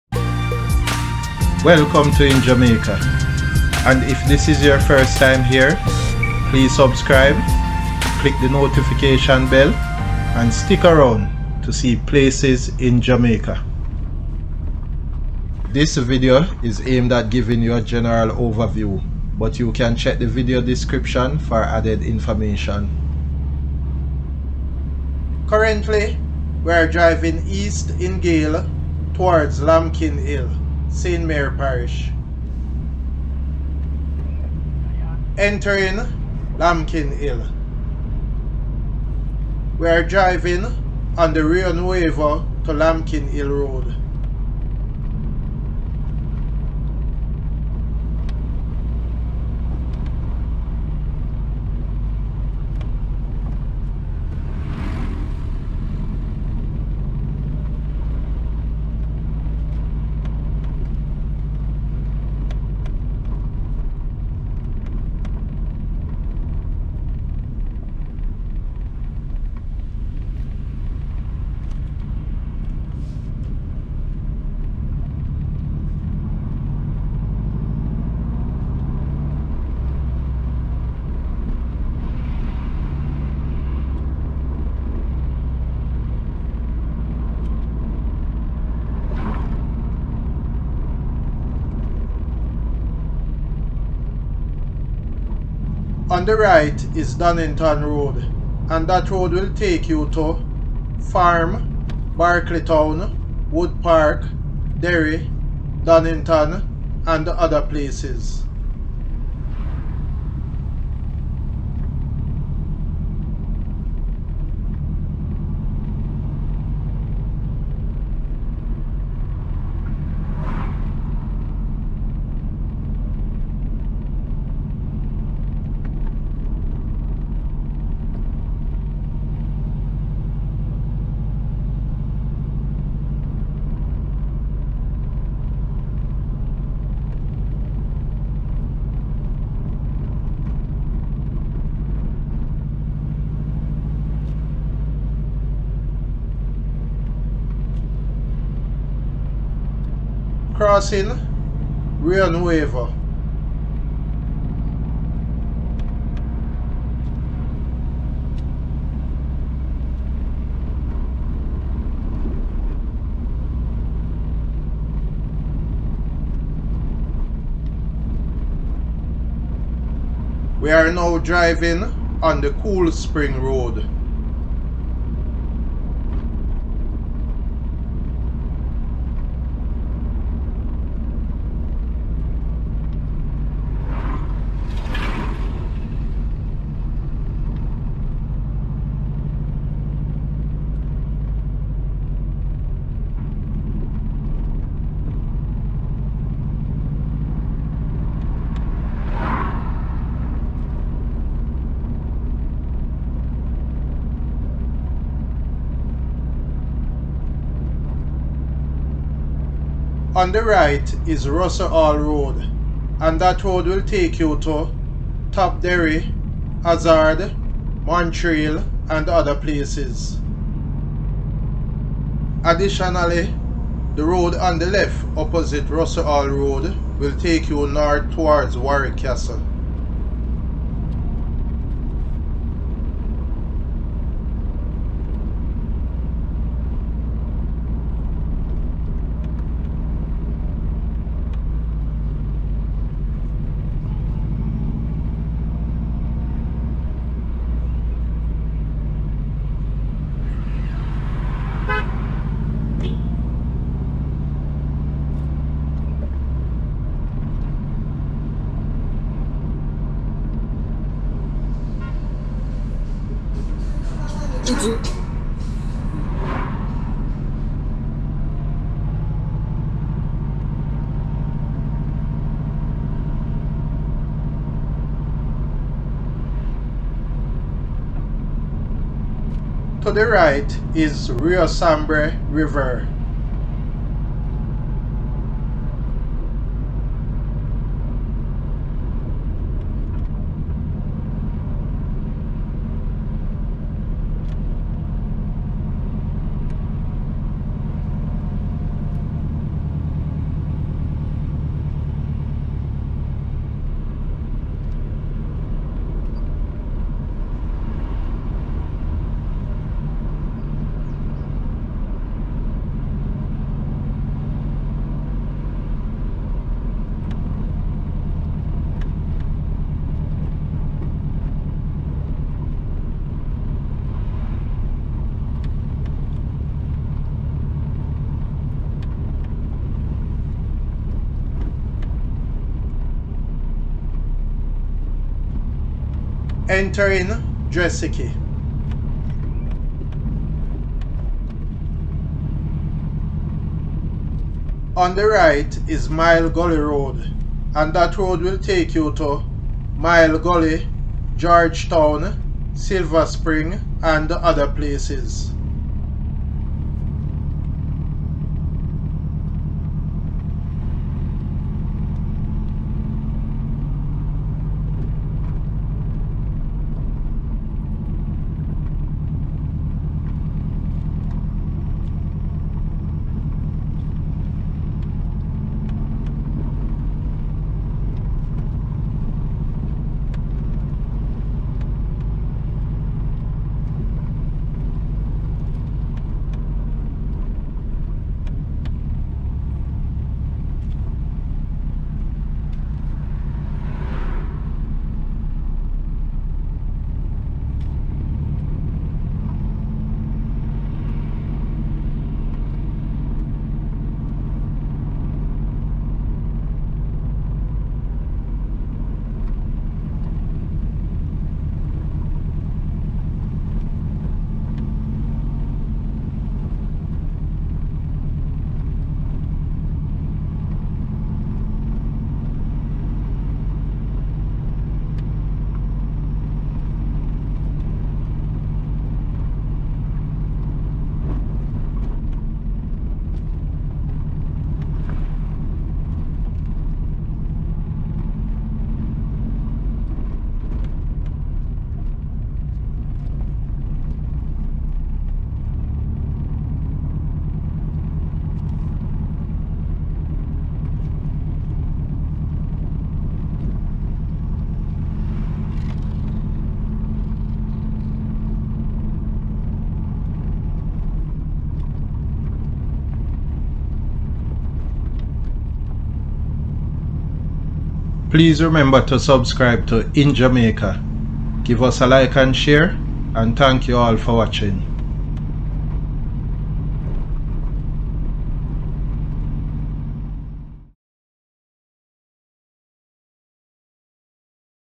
Lambkin Hill, St Mary, Jamaica
Lambkin Hill is located in St Mary, Jamaica (Caribbean).

⭐ Points of interest:
0:00 Intro
0:25 Gayle
0:35 Lambkin Hill
0:39 B13 - Rio Nuevo to Lambkin Hill Road
1:35 Donnington Road
1:38 To Farm
1:42 To Barclay Town
1:43 To Wood Park
1:44 To Derry
1:45 To Donnington
2:30 Rio Nuevo (River)
2:50 B13 - Cool Spring Road
3:30 Russell Hall Road
3:32 To Top Derry
3:36 To Hazard
3:37 To Montreal
3:42 To Warwick Castle
4:39 Rio Sambre (River)
5:36 Dressikie (also Dressikee, Dressike)
5:44 Mile Gully Road
5:47 To Mile Gully
5:51 To George Town
5:52 To Silver Spring
6:36 Cool Spring

LOCATION:
2023 Jamaica Road Trip